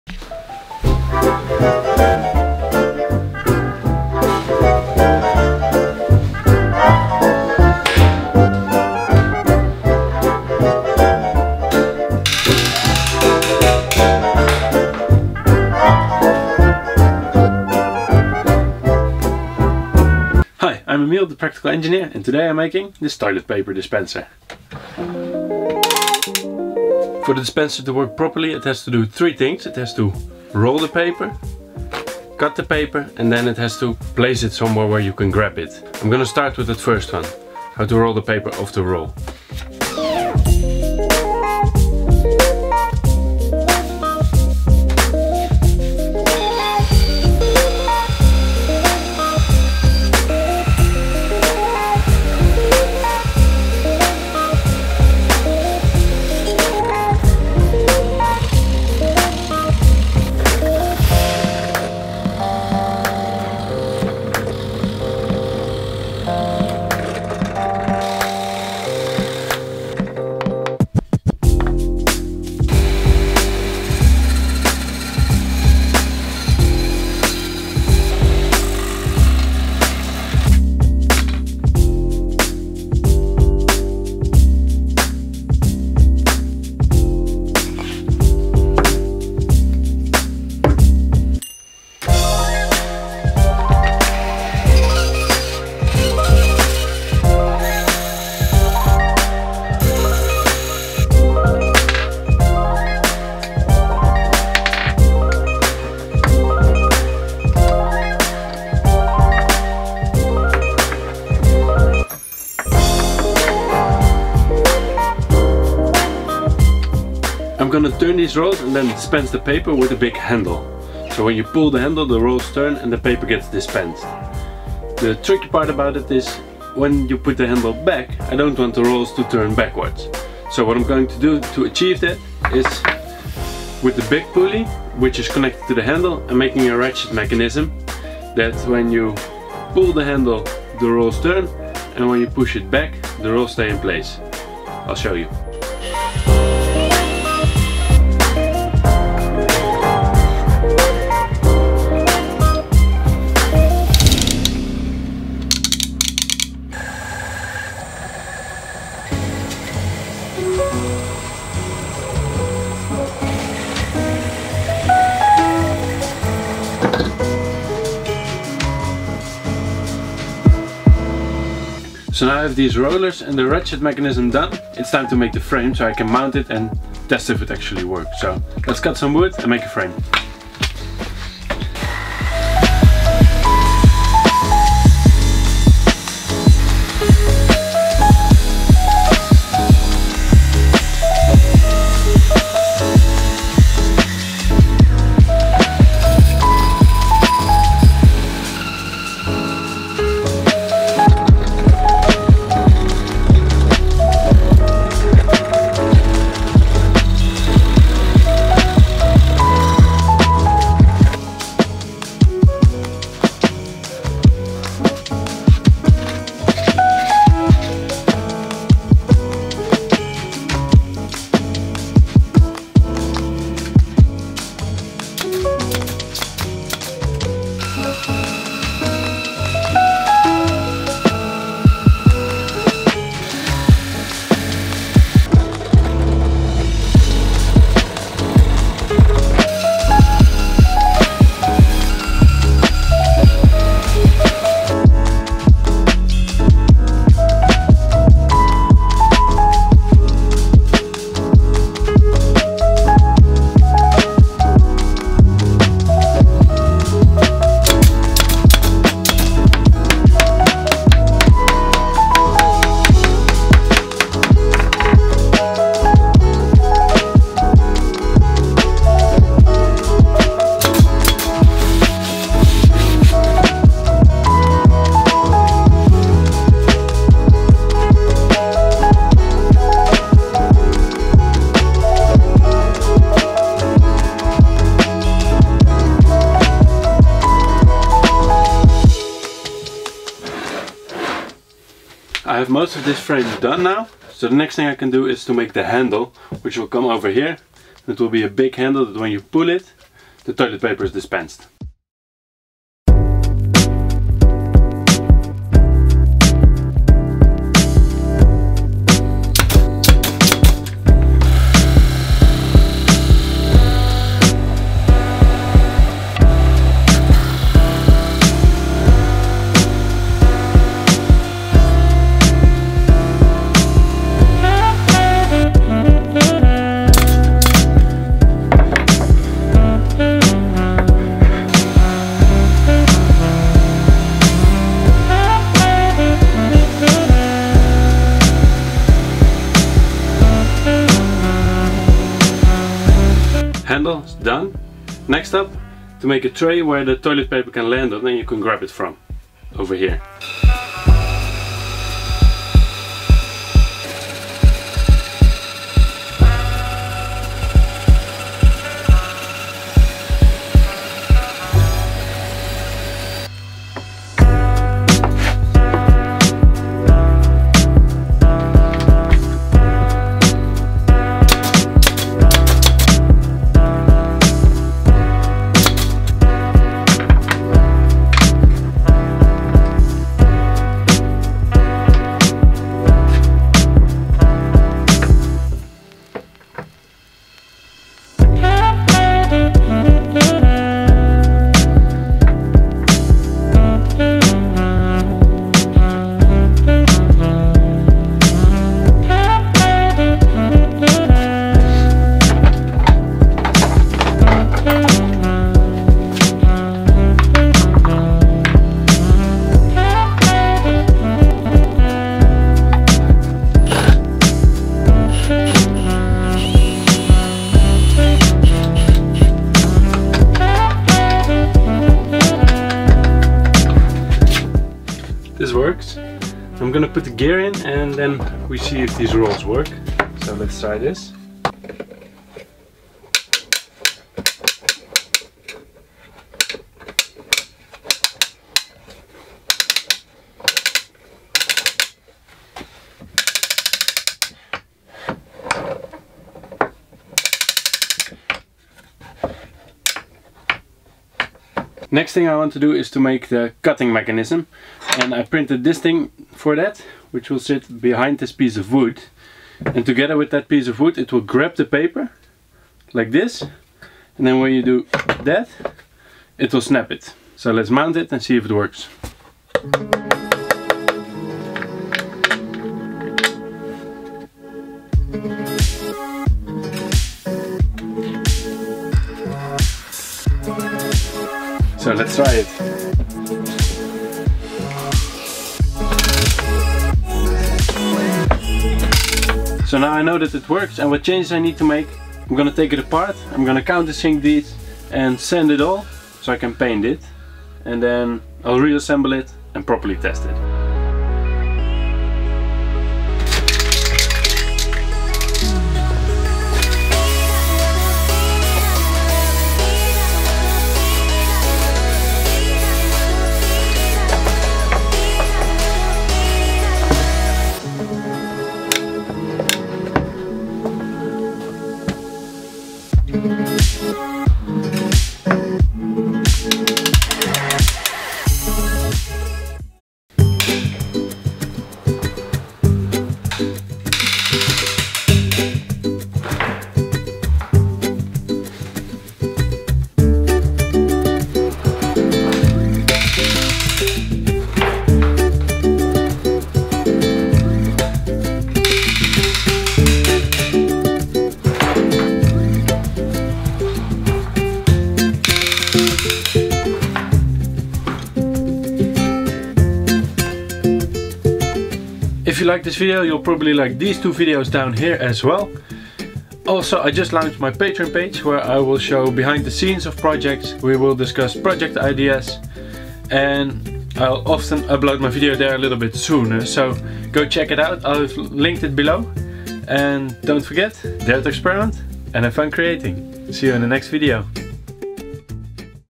One hand toilet paper machine - Automatic toilet paper dispenser
This week I made this fully mechanical toilet paper dispenser. I set the goal for myself to make a toilet paper dispenser that could dispense and cut two sheets of toiletpaper fully mechanical. After a couple of concepts I decided on the large handle with the ratchet mechanism to dispense the toilet paper and cut it.